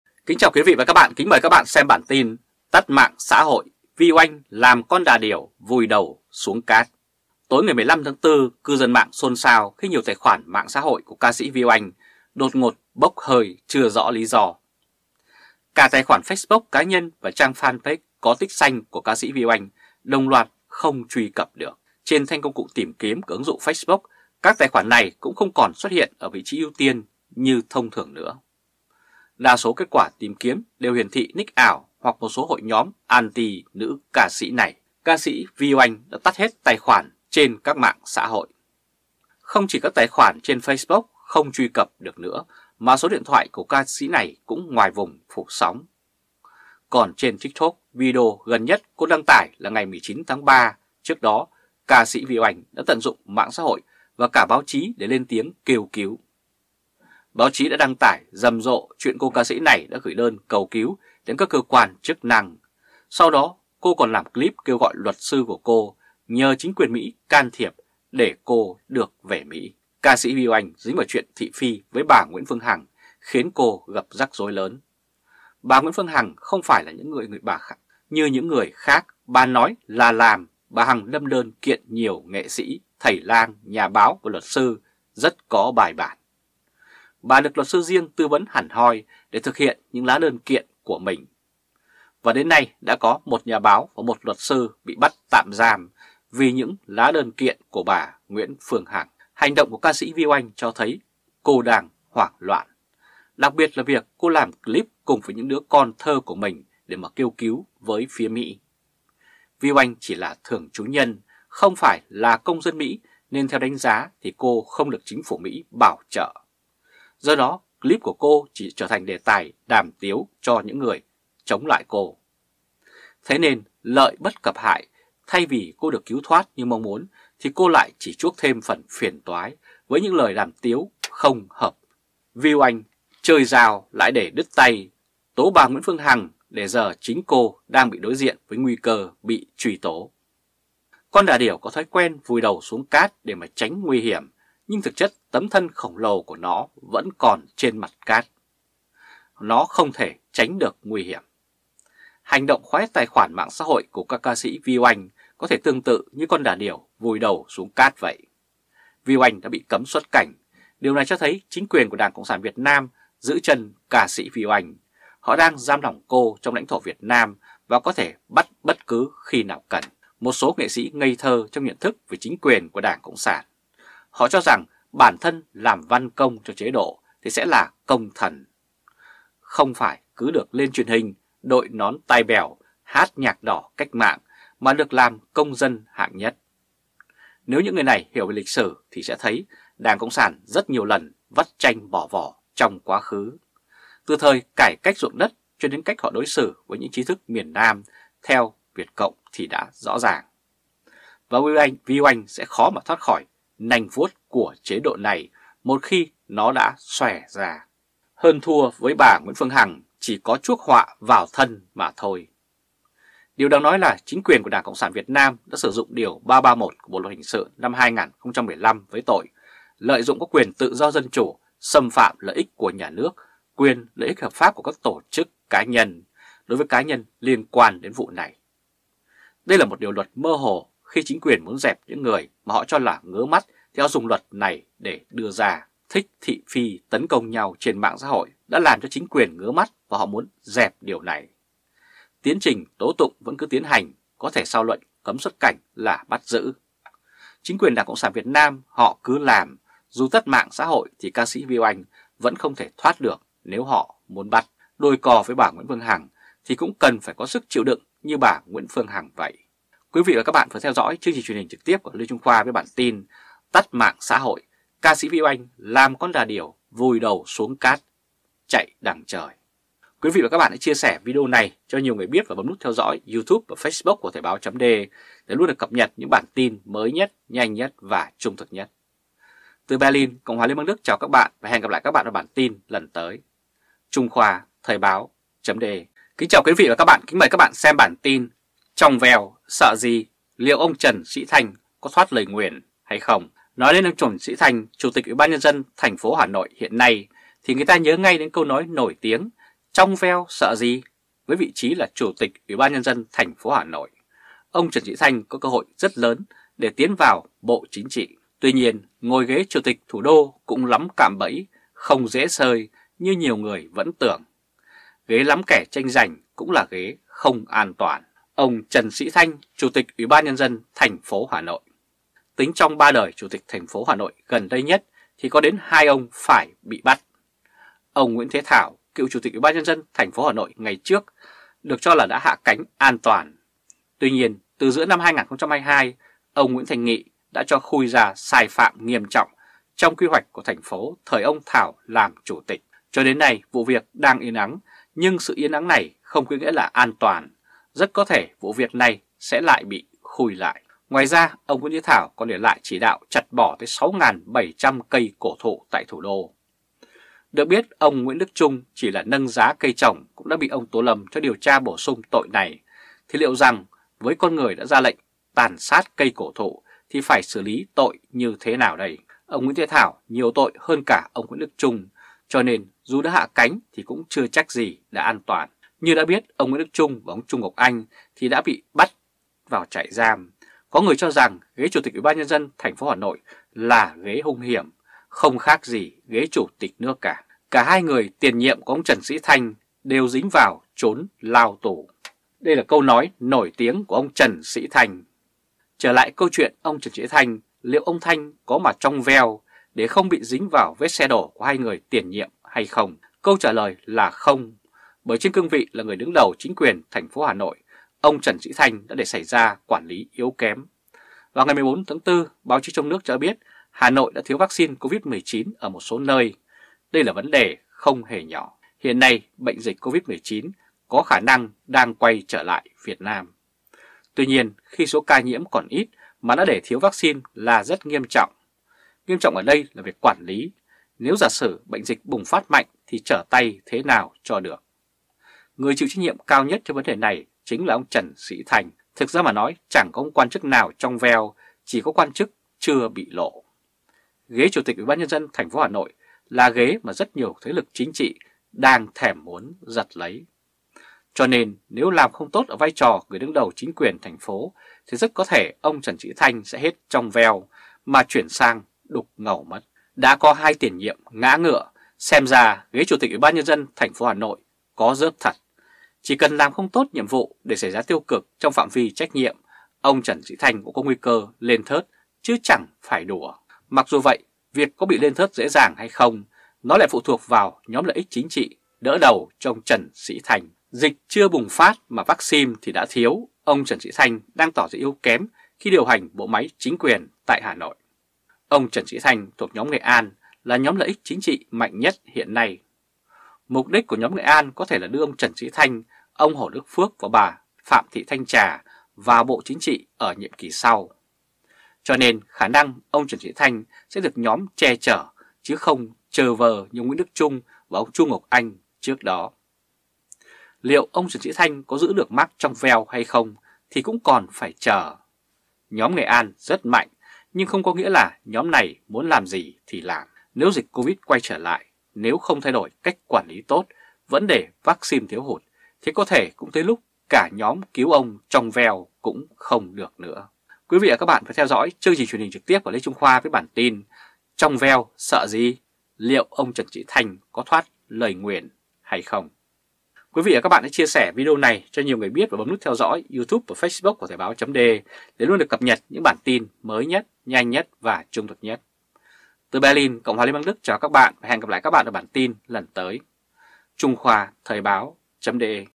Tắt mạng xã hội, Vy Oanh làm “con đà điểu vùi đầu xuống cát”. Chạy đằng trời!
Tiến trình tố tụng vẫn cứ tiến hành, có thể sau lệnh cấm xuất cảnh là bắt giữ. Chính quyền Cộng sản họ cứ làm, dù tắt mạng xã hội thì ca sĩ Vy Oanh vẫn không thể thoát được nếu họ muốn bắt. Đôi co với bà Nguyễn Phương Hằng thì cũng cần phải có sức chịu như bà Nguyễn Phương Hằng vậy.